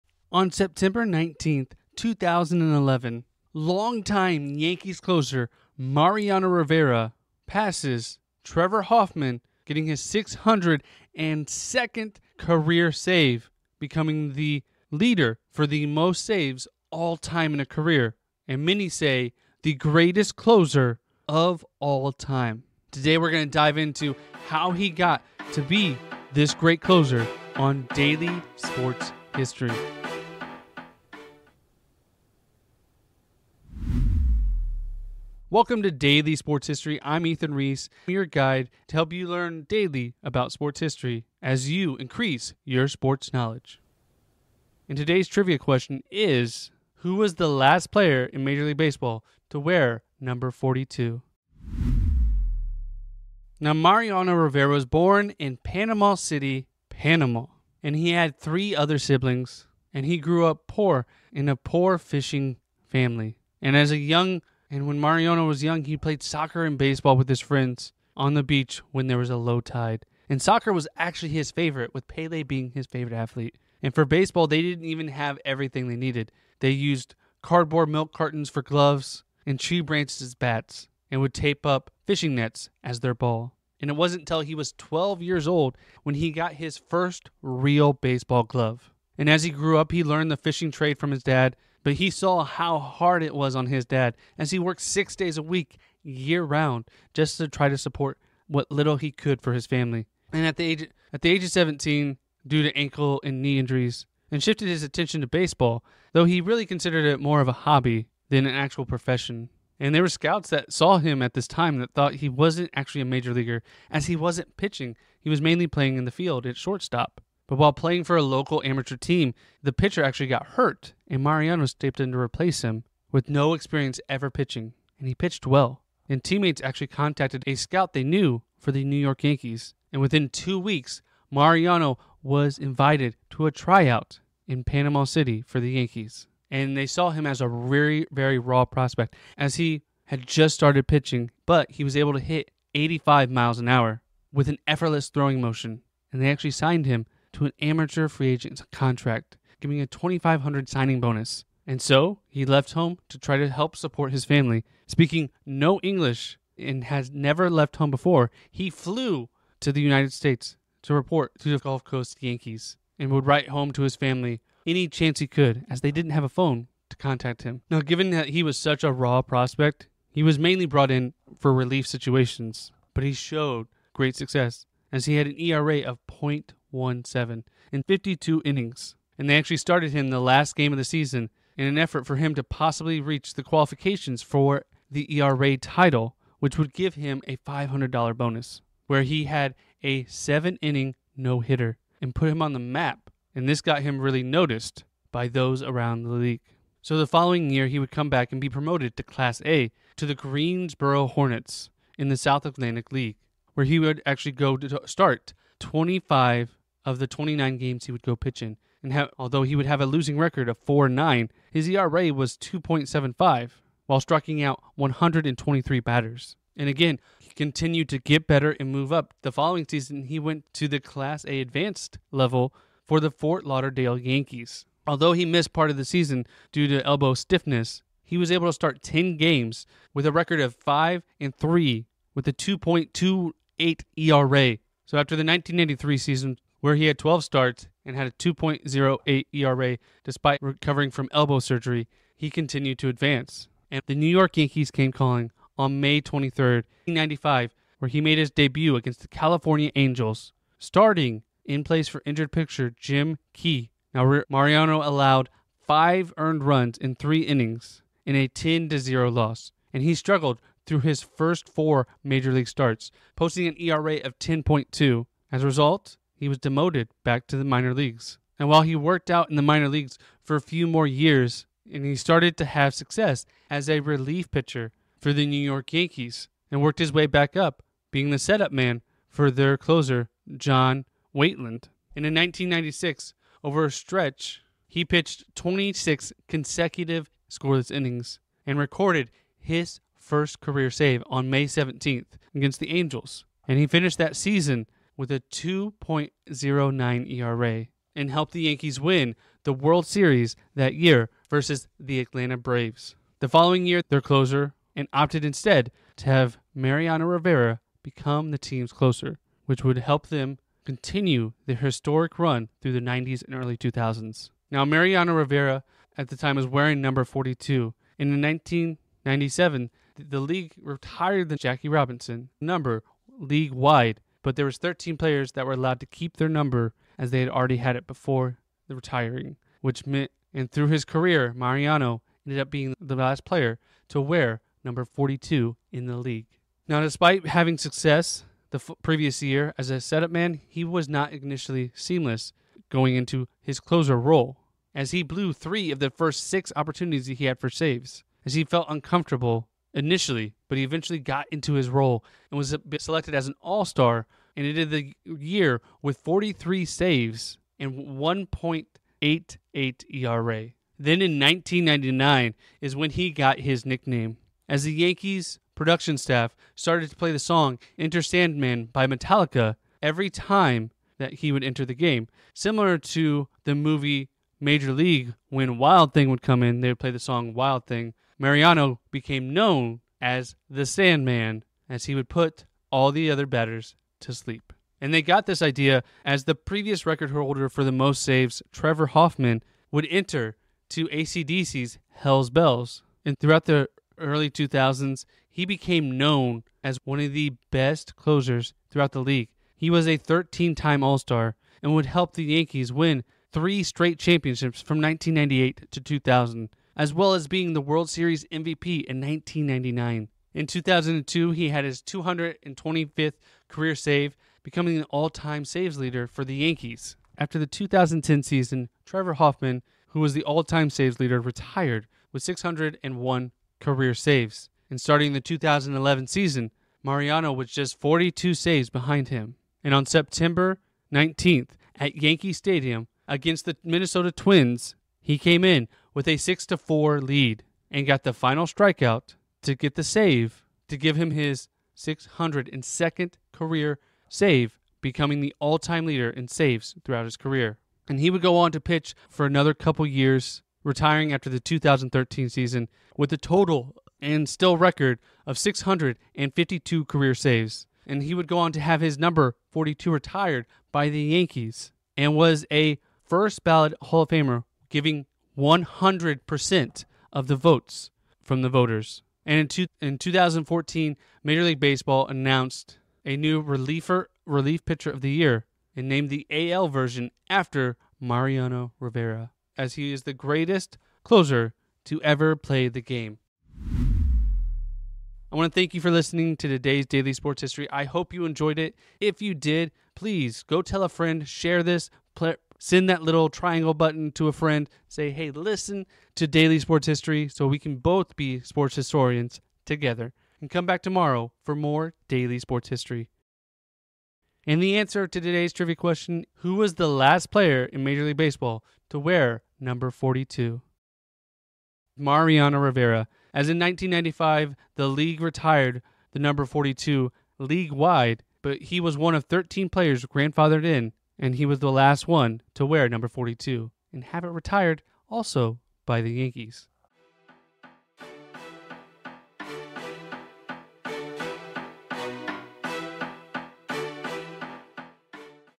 Mariano Rivera: The Closer Who Made History
Join us on Daily Sports History as we honor Mariano Rivera, the greatest closer in MLB history. Explore his legendary career with the New York Yankees, his record-breaking saves, and the impact he had on the game. From his iconic cutter to his Hall of Fame induction, discover why Rivera's legacy will forever be etched in baseball history.